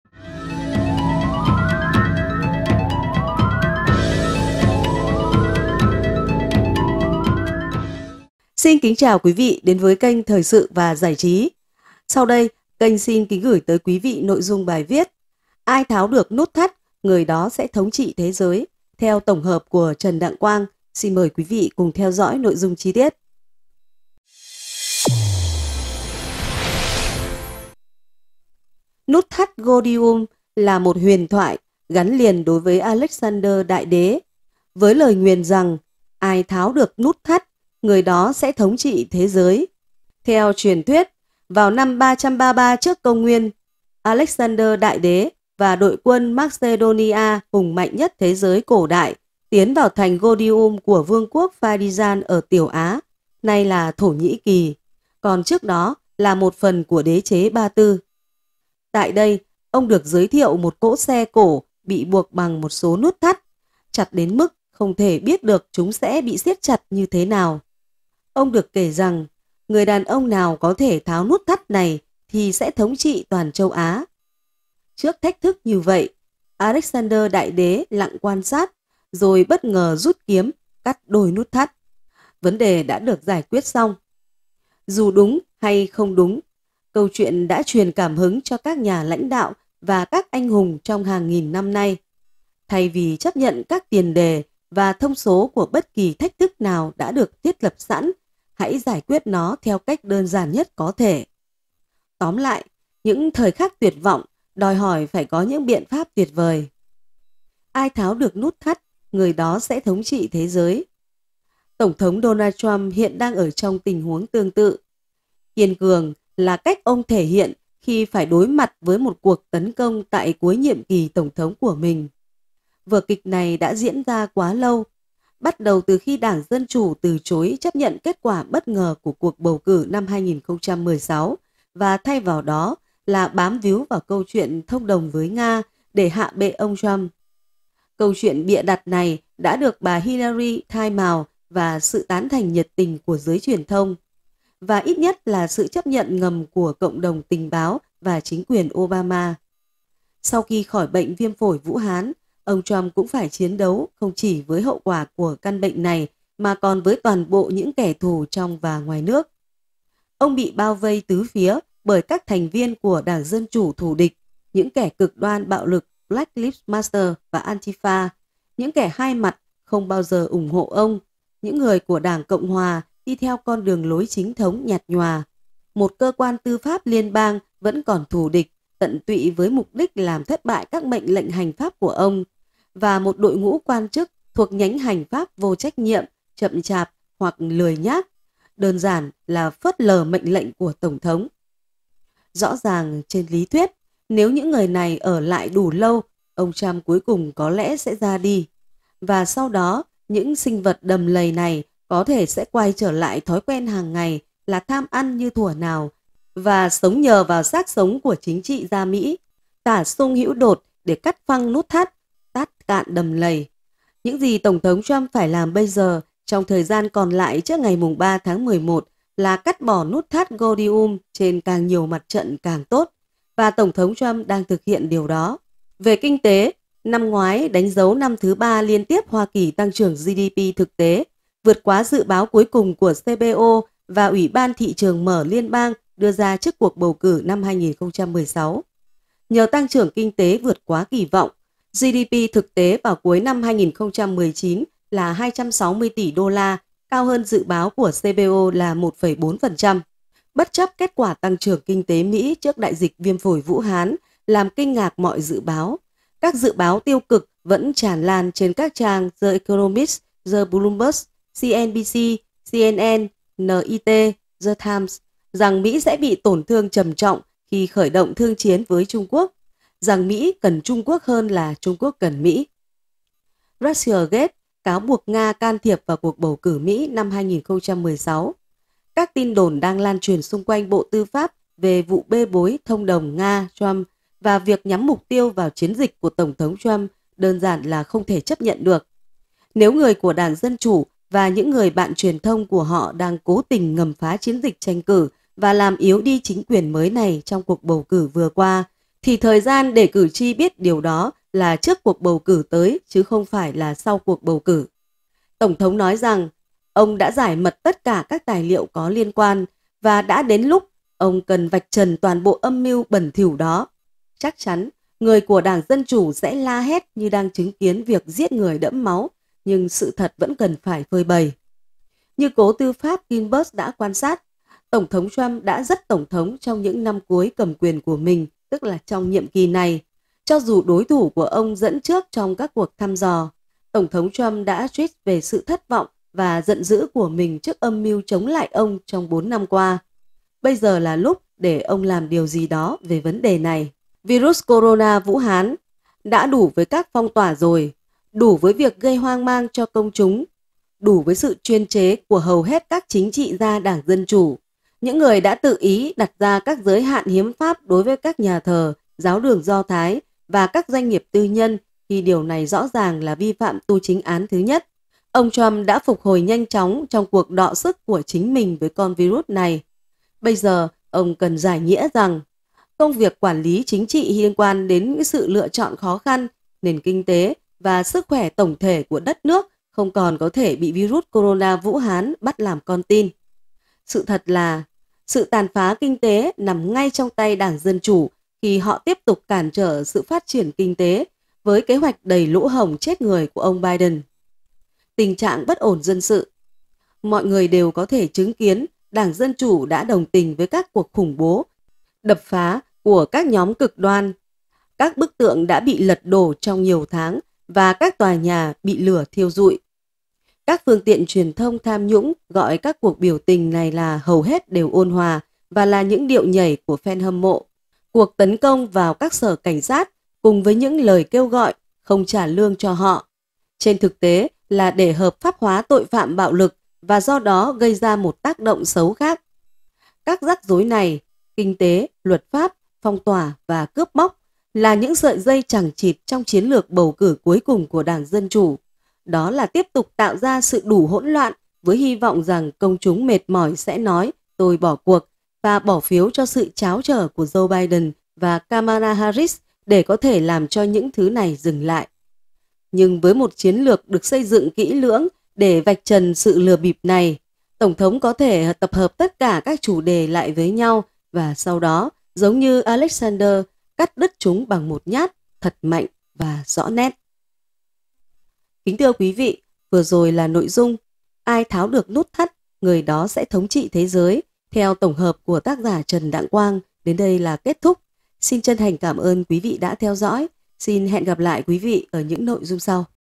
Ai tháo được nút thắt người đó sẽ thống trị thế giới!
# Nút thắt Gordium là một huyền thoại gắn liền với Alexander Đại đế. Với "lời nguyền" rằng "ai tháo được nút thắt; người đó sẽ thống trị thế giới"...
Theo truyền thuyết; vào năm 333 trước Công Nguyên; Alexander Đại đế và đội quân Macedonia hùng mạnh nhất thế giới cổ đại tiến vào thành Gordium của vương quốc Phyrigian ở Tiểu Á; nay là Thổ Nhĩ Kỳ; còn trước đó là một phần của đế chế Ba Tư.
Tại đây; ông được giới thiệu một cỗ xe cổ bị buộc bằng “một số nút thắt; chặt đến mức không thể biết được chúng bị siết chặt như thế nào”. Ông được kể rằng người đàn ông nào có thể tháo nút thắt này thì sẽ thống trị toàn châu Á.
Trước thách thức như vậy; Alexander Đại đế lặng quan sát; rồi bất ngờ rút kiếm cắt đôi nút thắt. Vấn đề đã được giải quyết xong.
Dù đúng hay không; câu chuyện đã truyền cảm hứng cho các nhà lãnh đạo và các anh hùng trong hàng nghìn năm: Thay vì chấp nhận các tiền đề và thông số của bất kỳ thách thức nào đã được thiết lập sẵn; hãy giải quyết nó theo cách đơn giản nhất có thể.
Tóm lại; những thời khắc tuyệt vọng đòi hỏi những biện pháp tuyệt vời.
Tổng thống Donald Trump hiện đang ở trong tình huống tương tự. “Kiên cường" là cách ông thể hiện khi phải đối mặt với cuộc tấn công tại cuối nhiệm kỳ tổng thống của mình.
Vở kịch này đã diễn quá lâu; bắt đầu khi đảng Dân chủ từ chối chấp nhận kết quả bất ngờ của cuộc bầu cử năm 2016 và thay vào đó bám víu vào câu chuyện "thông đồng với Nga" để hạ bệ ông Trump.
Câu chuyện bịa đặt này đã được bà Hillary Clinton khai mào với sự tán thành nhiệt tình của giới truyền thông và ít nhất là sự chấp thuận ngầm của Cộng đồng Tình báo và chính quyền Obama.
Sau khi khỏi bệnh viêm phổi Vũ Hán; ông Trump cũng phải chiến đấu không chỉ với hậu quả của căn bệnh này mà còn với toàn bộ những kẻ thù trong và ngoài nước.
Ông bị bao vây tứ phía bởi các thành viên đảng Dân chủ thù địch; những kẻ cực đoan bạo lực Black Lives Matter và Antifa; những kẻ hai mặt không bao giờ ủng hộ ông; những người của đảng Cộng hòa theo đường lối chính thống nhạt nhòa; một cơ quan tư pháp liên bang vẫn còn thù địch - “tận tụy” với mục đích làm thất bại các mệnh lệnh hành pháp của ông; và một đội ngũ quan chức thuộc nhánh hành pháp vô trách nhiệm; chậm chạp hoặc lười nhác; đơn giản là phớt lờ mệnh lệnh của tổng thống.